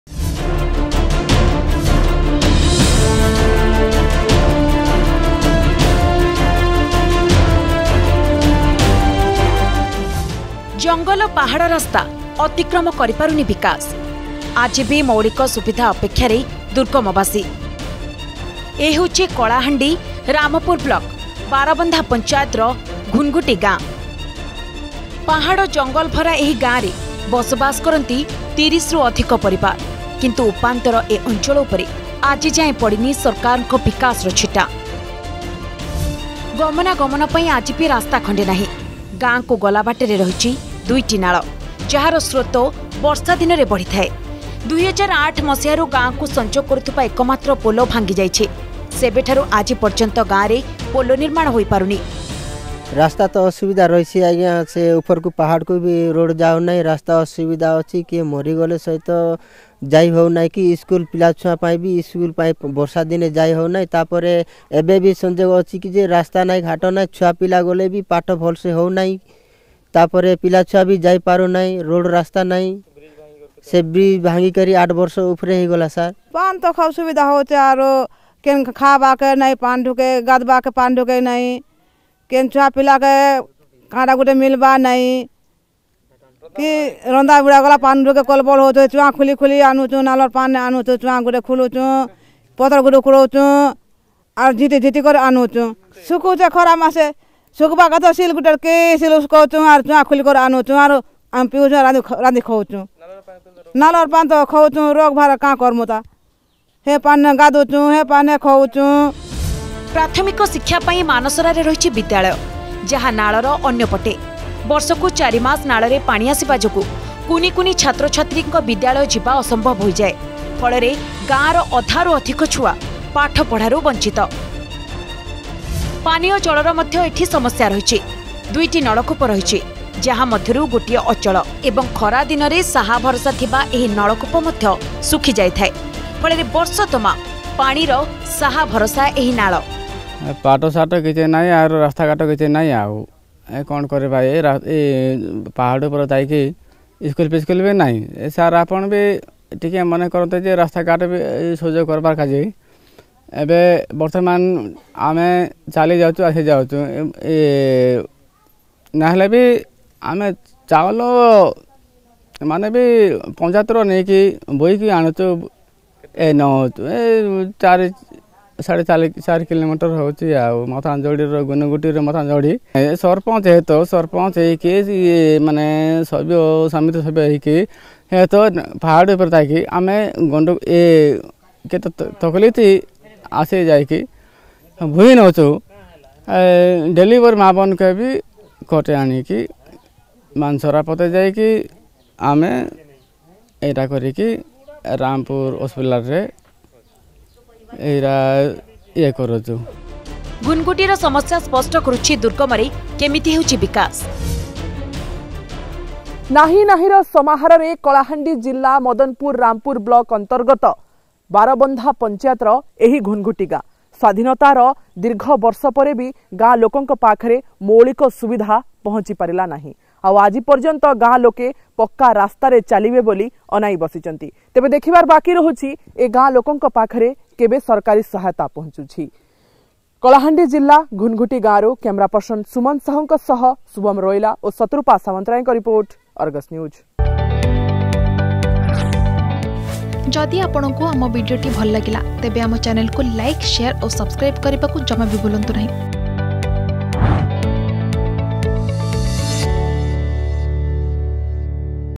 ମୈାଳିକ ସୁବିଧା ଅପେକ୍ଷାରେ ଦୁର୍ଗମବାସୀ || Basic Facilities
ନାହିଁ ନାହିଁରେ ଜୀବନ ବଞ୍ଚୁଛନ୍ତି ଘୁନଘୁଟି ଗ୍ରାମବାସୀ । ଜଙ୍ଗଲ ପାହାଡ ରାସ୍ତା ଅତିକ୍ରମ କରି ପାରୁନି ବିକାଶ। ଆଜିବି ମୈଳିକ ସୁବିଧା ଅପେକ୍ଷାରେ ଘୁନଘୁଟି ଗାଁ। ଆଜି ଯାଏଁ ପଡିନି ସରକାରଙ୍କ ବିକାଶର ଛିଟା।